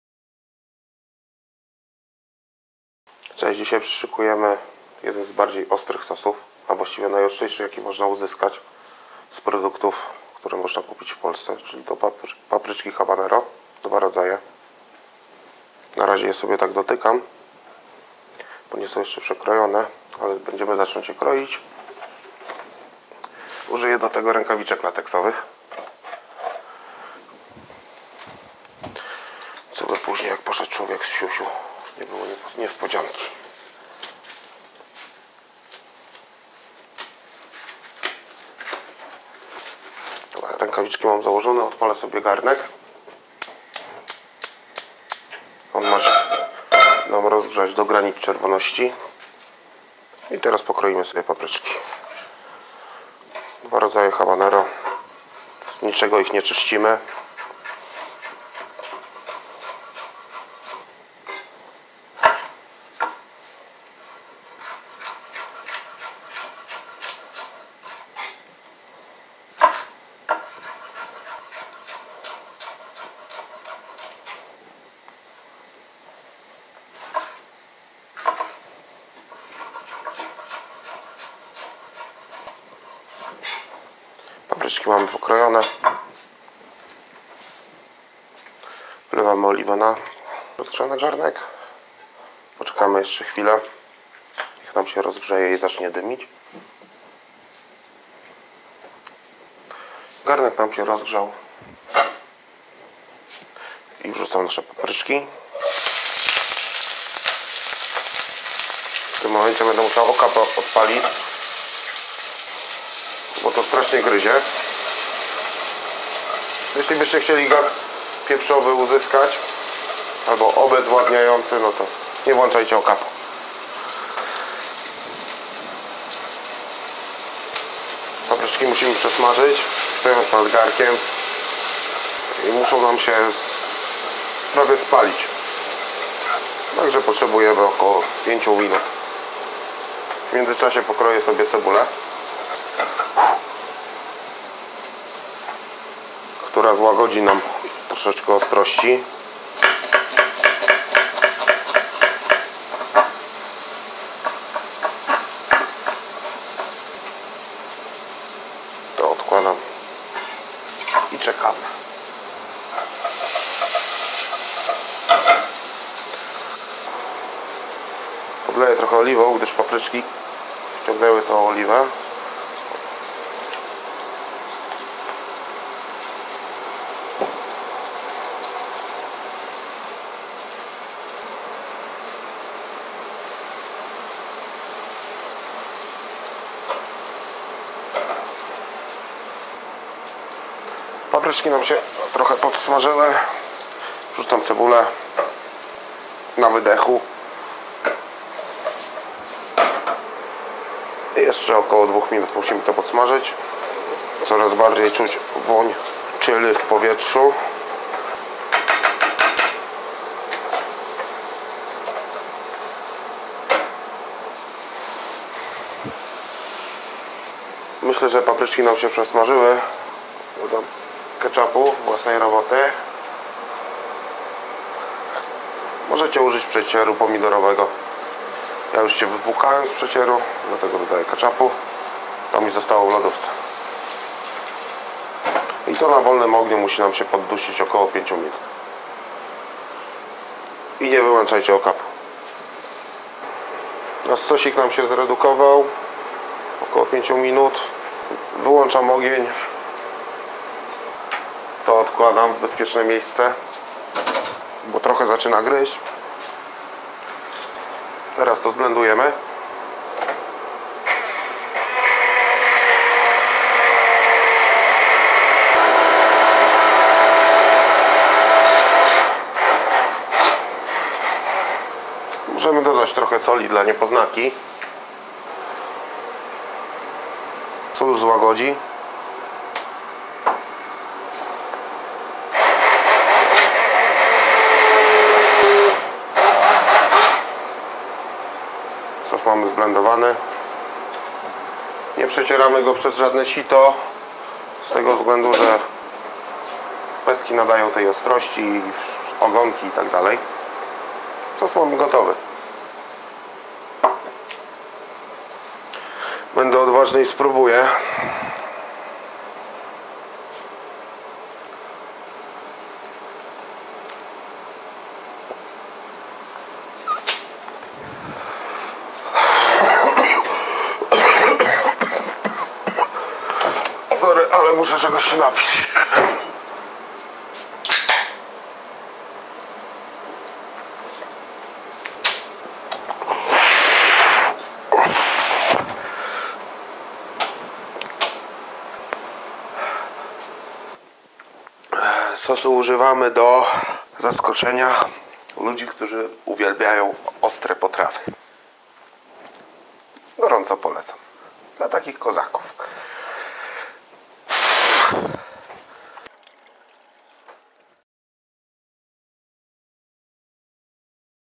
Najostrzejszy sos naturalny, Habanero, KitchenBook
KitchenBook Jak zrobić ostry sos habanero? Najostrzejszy sos naturalny.

Składniki:
papryczki habanero,
cebula,
sól,
rękawiczki lateksowe :-)